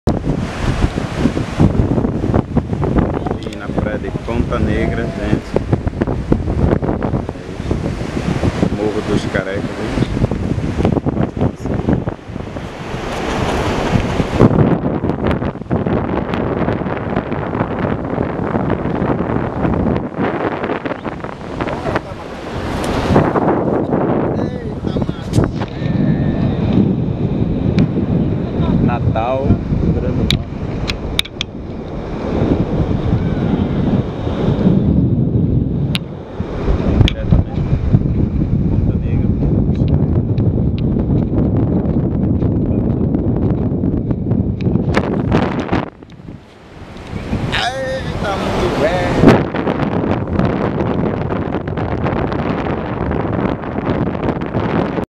PRAIA DE PONTA NEGRA -NATAL BRASIL
estou de ferias com a familia